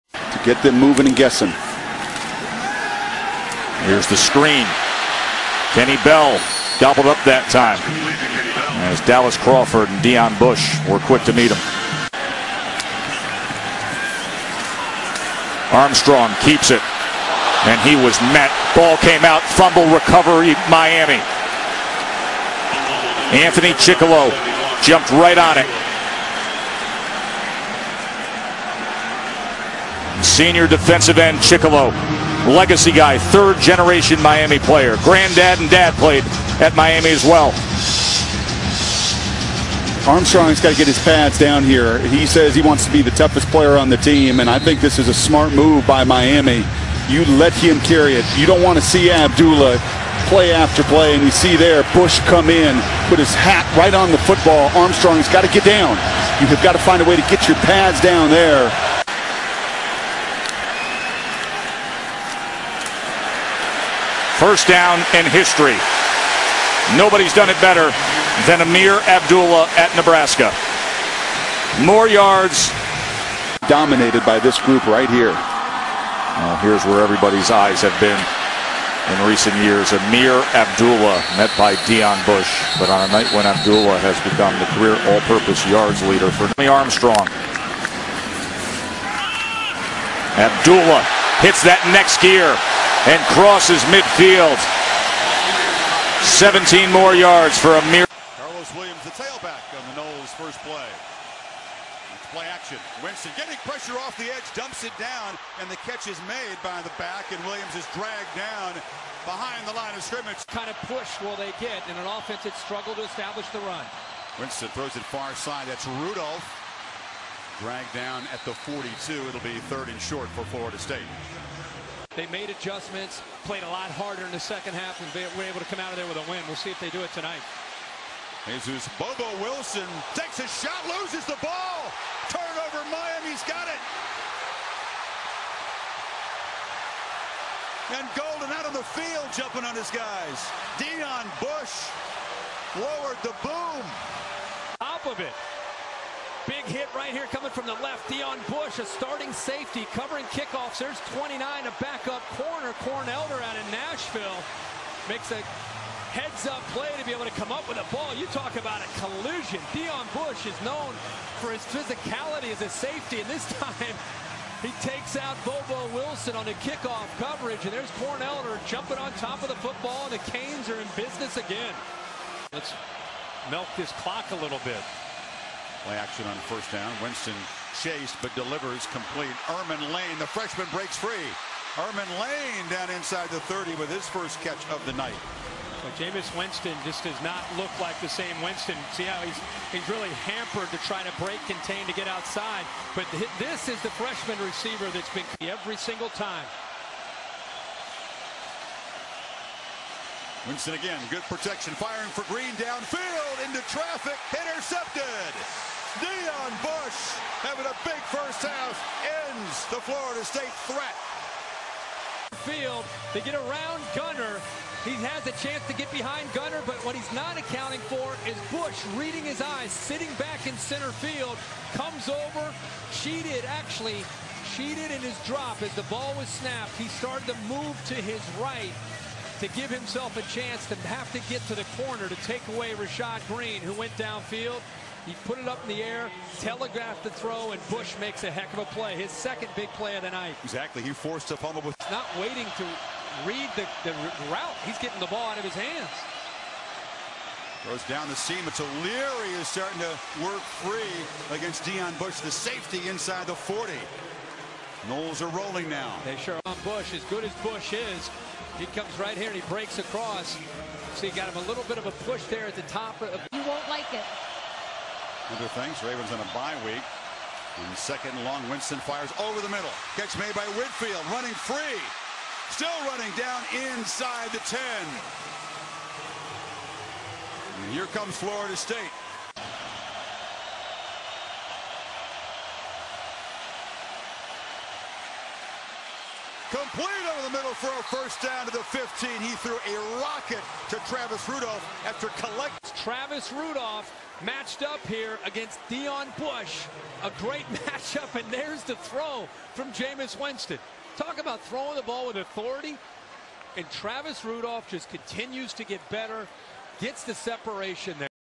Deon Bush - Miami Football - FS - 2014 Nebraska & FSU Games
Deon Bush Jr FS Miami 2014 Highlights
6'1" 198 lb. 2014: 12 GP 53 Tkl 4.0 TFL 2 Int 5 FF 3 PBU